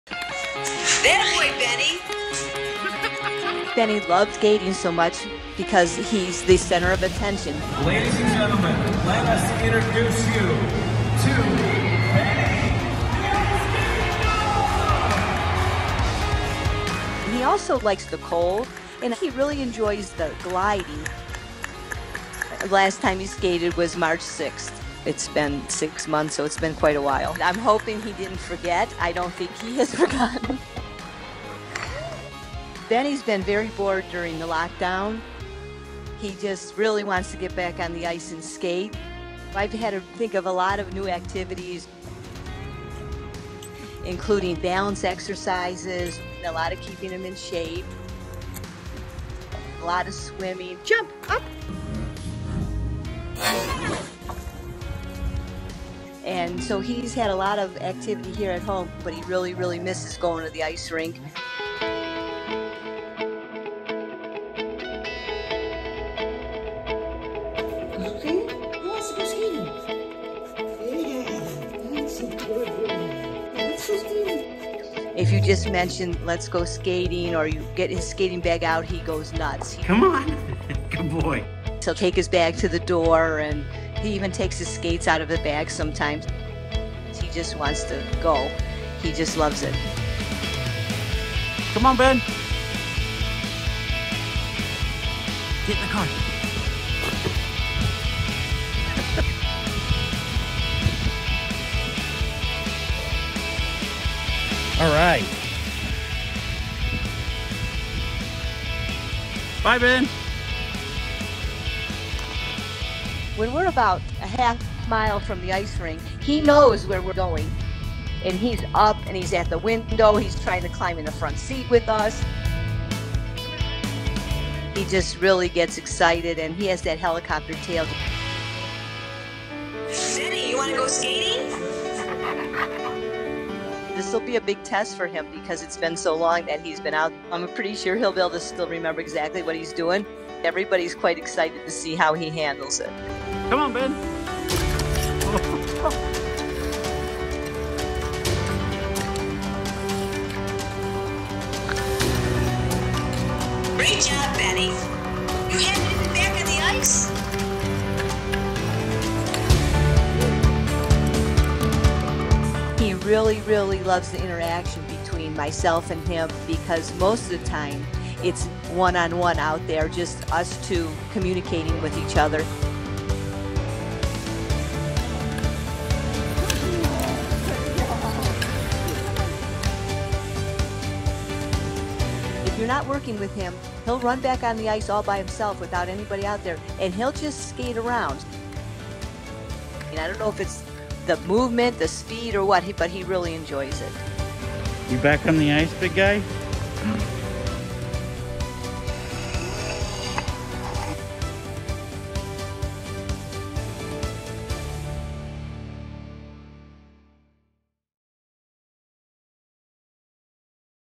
Dog Who’s Obsessed With Ice Skating Gets A Special Surprise After Quarantine | The Dodo
Dog obsessed with ice skating gets back on the ice for the first time after months of lockdown in Las Vegas.

You can keep up with Benny and all of his ice skating adventures on Instagram